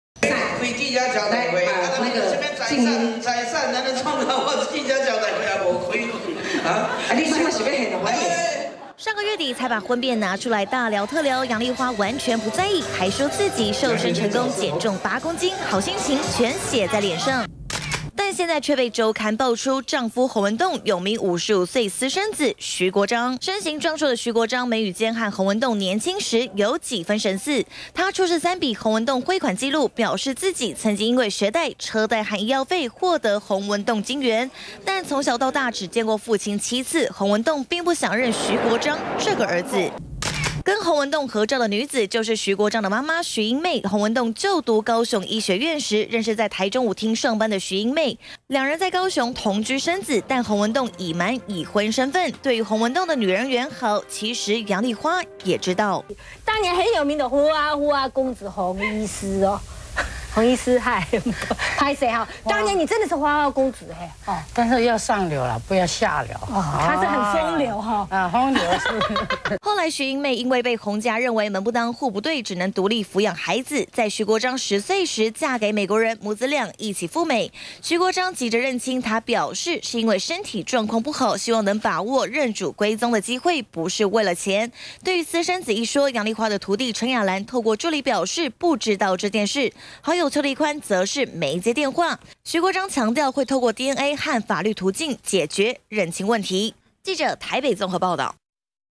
20160511中天新聞 週刊爆洪文棟有私生子 現身要求認祖歸宗
上個月底楊麗花與丈夫洪文棟，才因為週刊爆出分居傳聞，夫妻倆合照攜手闢謠，但現在又有名長期居住美國的男子 徐國璋，出面向週刊表示，自己是洪文棟的私生子。他表示母親當年在台中舞廳上班，認識當時就讀高雄醫學院的父親，兩人同居生子，但母親身份卻不被洪家人接受。最後在他10歲時，嫁給美國人 母子倆一起赴美。會突然想認親，是因為自己最近身體狀況不好，想把握認祖歸宗的機會。